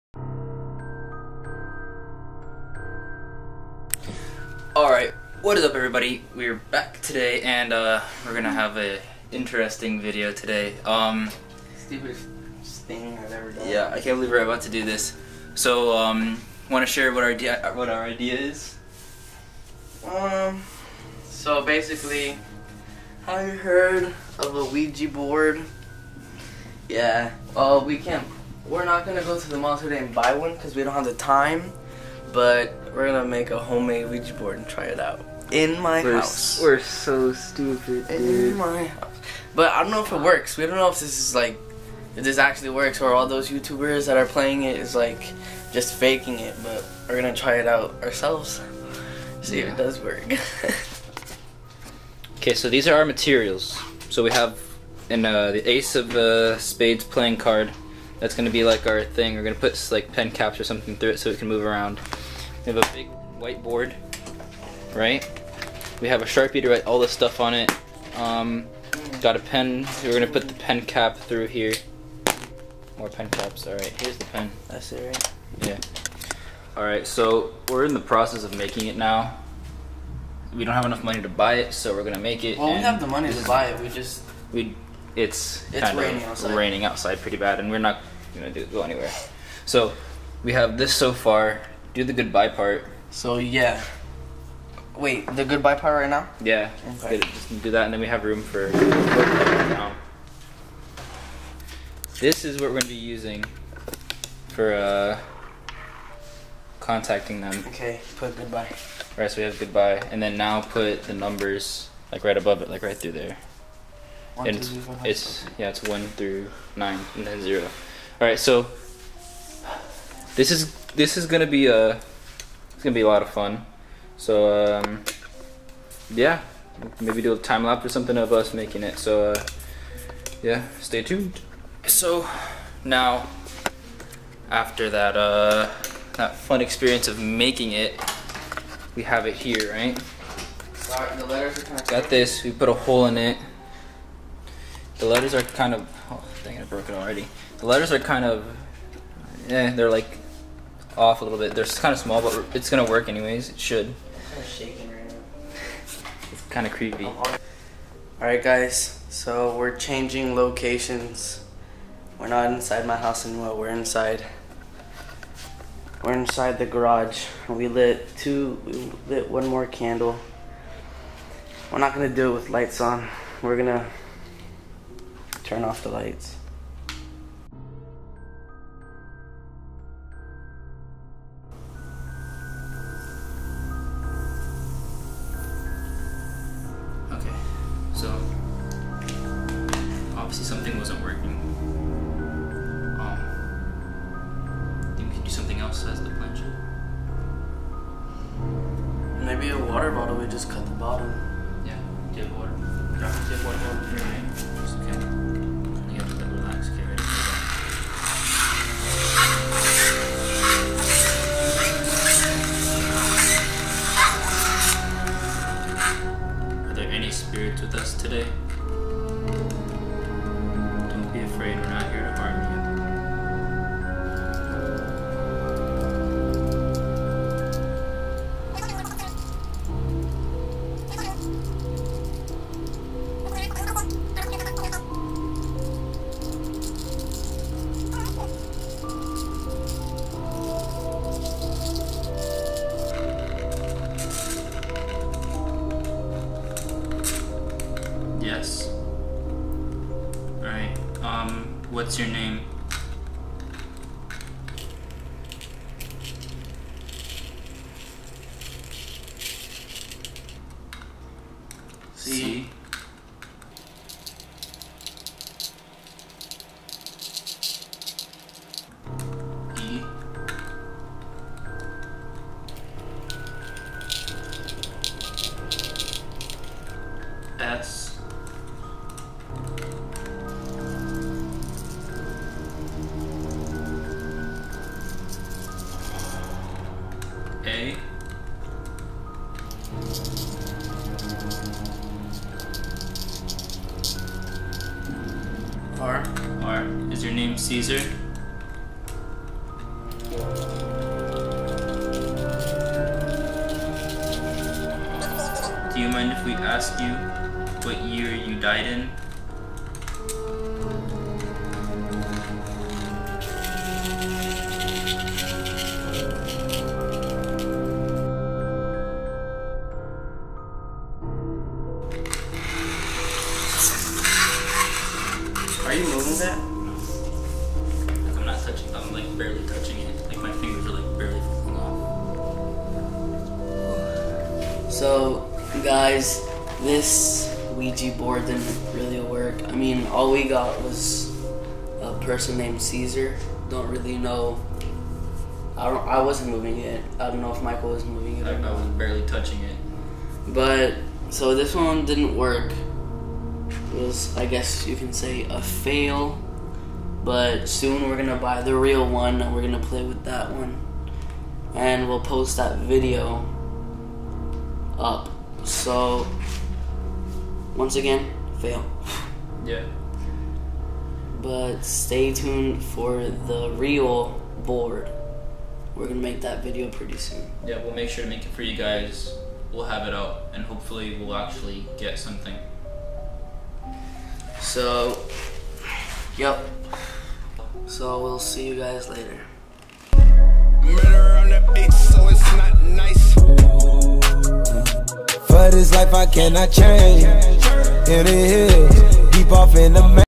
PLAYING WITH A HOMEMADE OUIJA BOARD
Today we made a ouija board. Please do not recreate what we did in this video. We were lucky to not interact with any evil spirits.